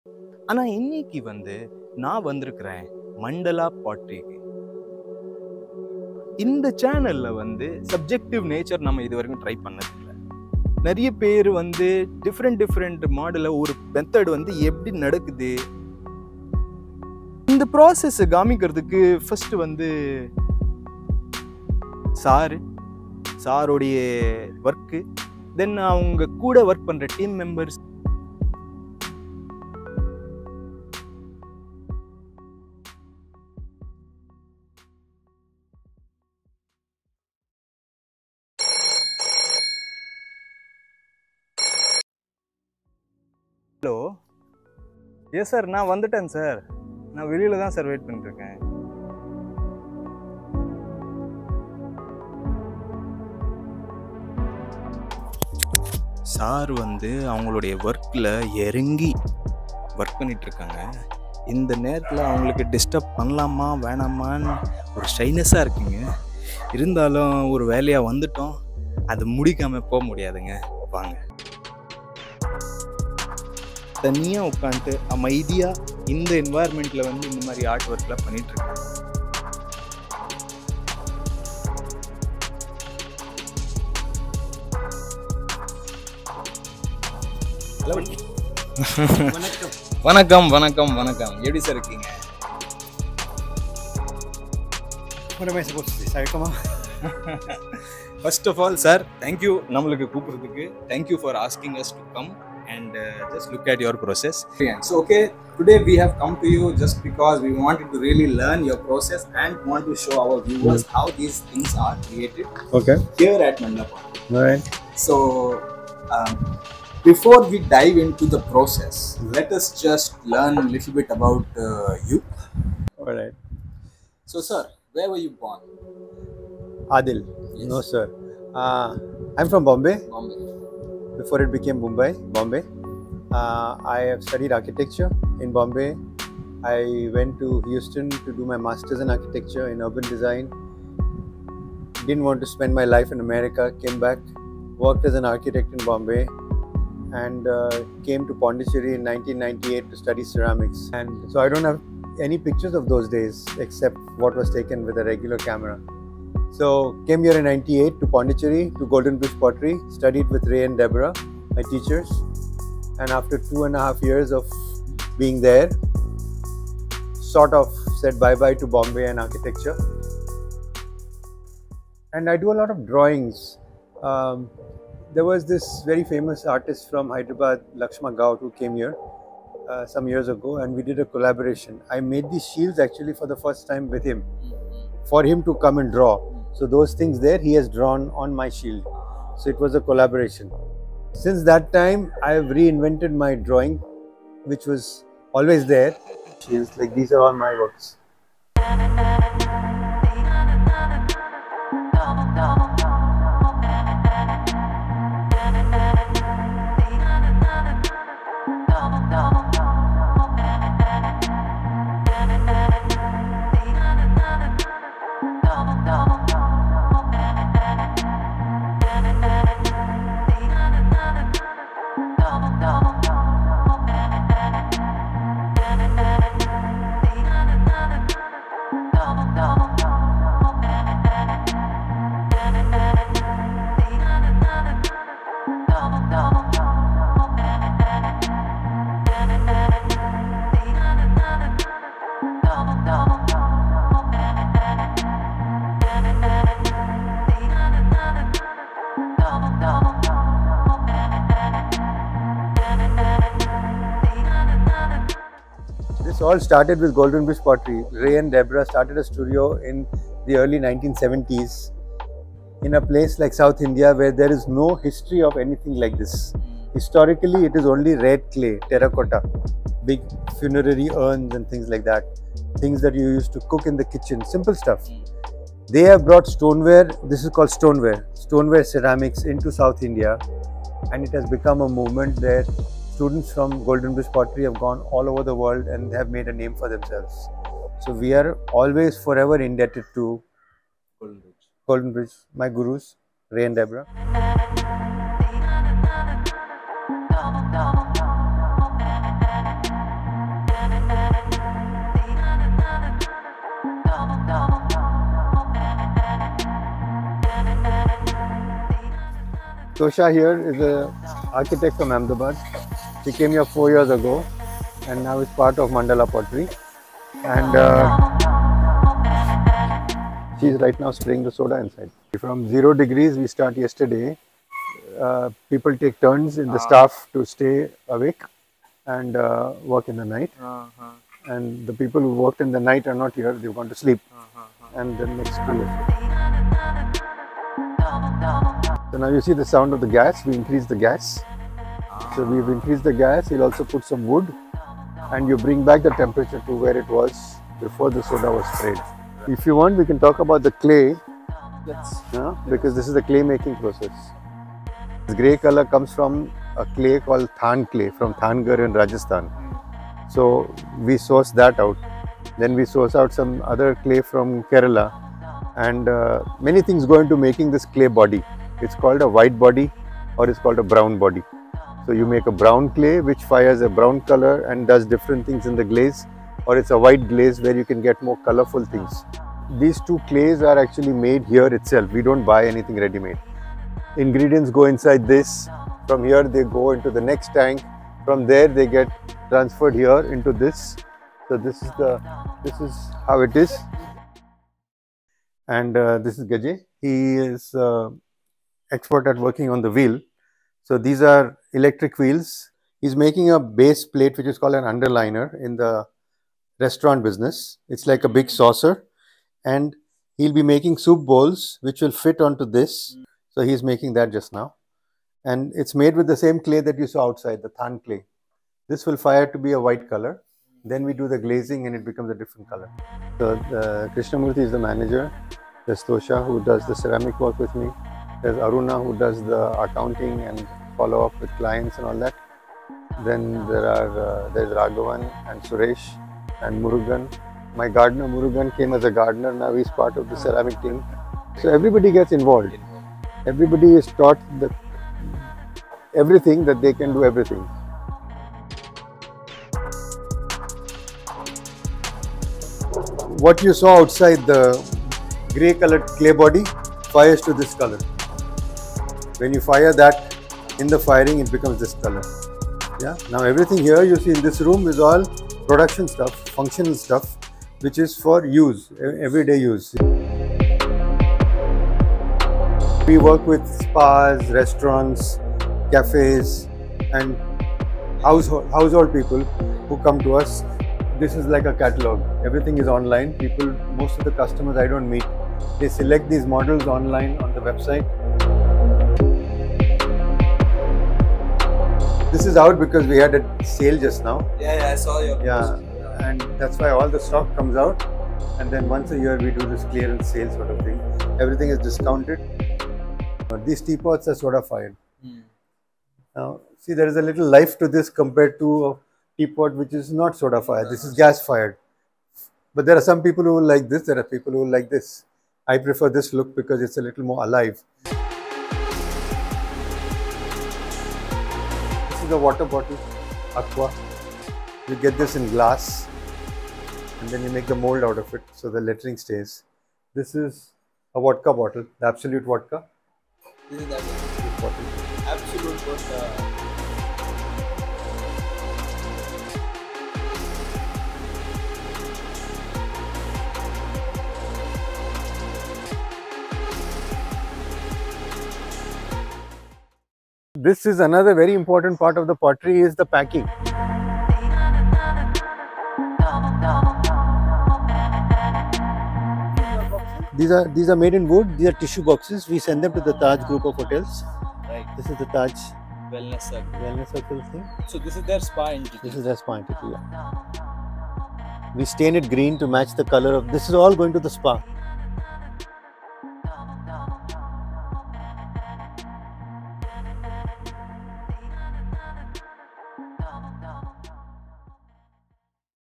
Mandala Pottery Auroville with Adil Writer and team.
Join me in this exclusive sneak peek in the world of Mandala Pottery with Adil Writer.

“Mandala Pottery is a ceramic studio in Auroville making functional tableware, architectural ceramics and sculpture which is fired with wood, soda or gas.”

A world renowned ceramic artist Adil shares with Goodman Pondy his incredible journey, vision and the relentless workforce that propels the Mandala Pottery of today.

Mandala pottery an Auroville unit has been excelling and setting an example for last 20+ years.

Please share your thoughts and wishes in the comments 😇